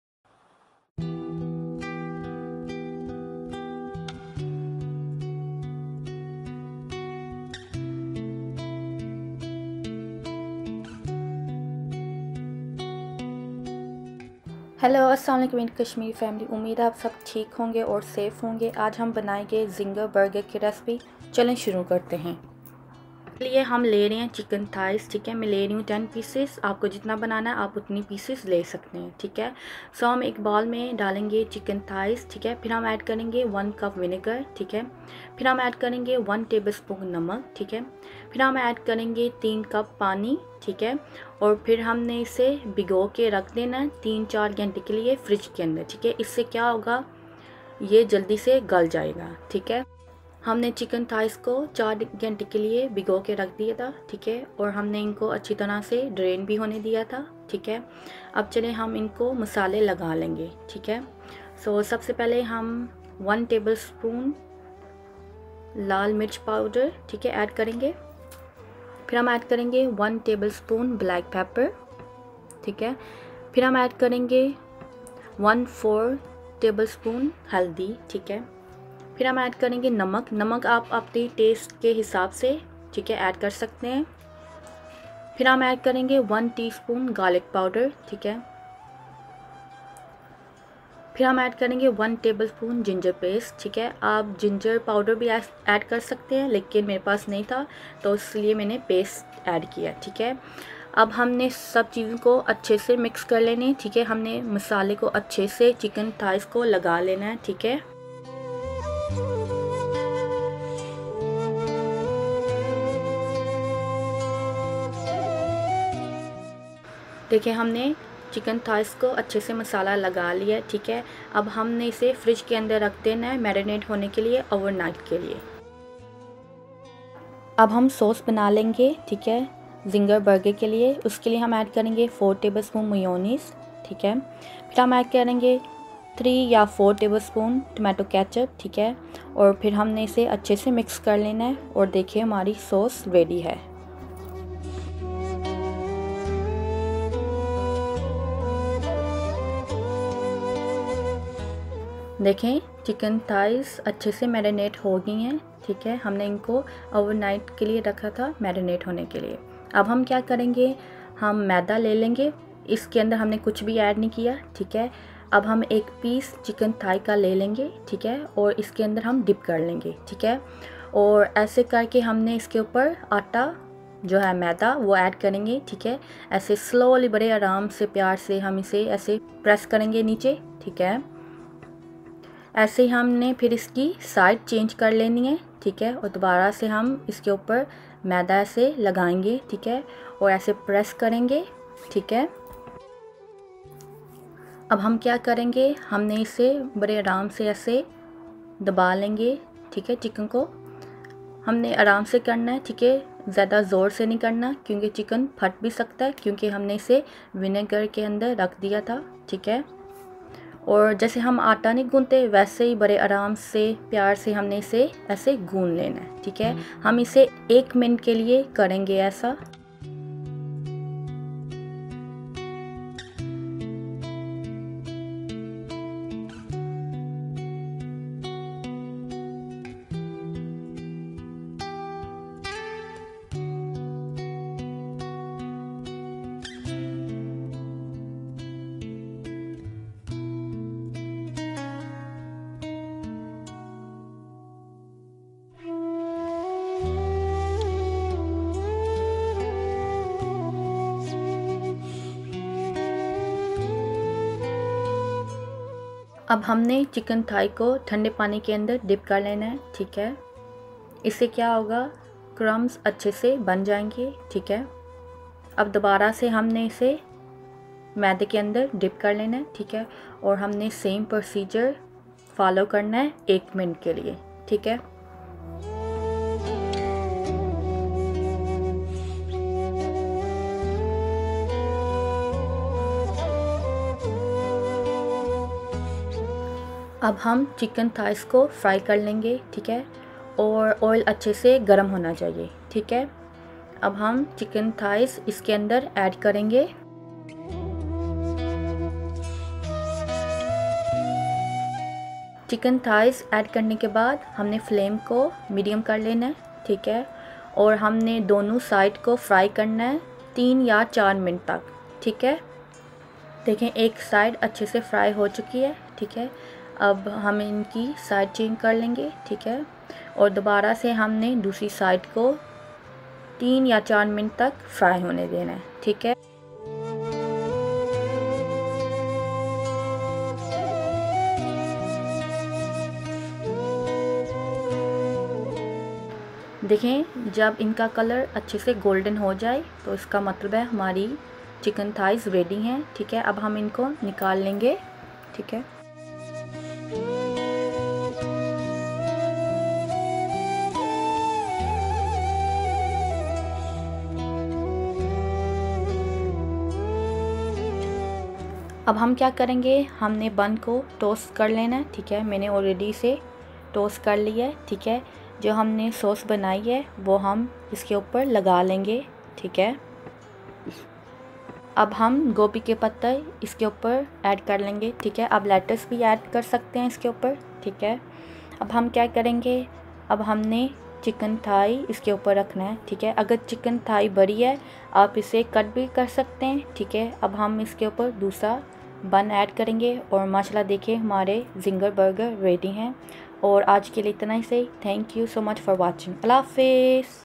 crispy chicken burger recipe - kashmiri kitchen with kiran
Ingredients:
To soak Chicken Thighs you will need:
1 cup of Vinegar
1 tbsp Salt
For Masala:
Chicken Thighs
1 tbsp of Black pepper
1 tbsp of Red chili powder
1/4 Turmeric powder
Salt per taste
1 tbsp of ginger powder
1 tbsp of Garlic paste
For coating:
Plain purpose
Coldwater
Oil for frying
For topping:
Buns
4 tbsp of Mayonnaise
4 tbsp of tomato ketchup
Cabbage/Lecture

THANK YOU FOR ALWAYS TUNING IN. ❤️